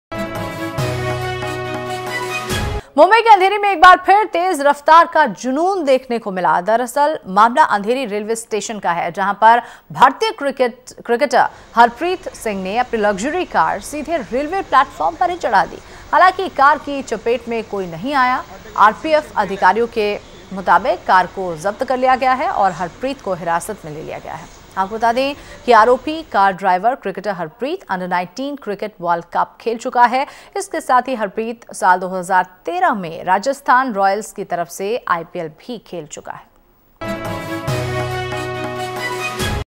A cricketer climb his speeding luxury car on the railway platform in Mumbai
A cricketer Harpteet Singh climb his speeding luxury car on Andheri railway platform in Mumbai...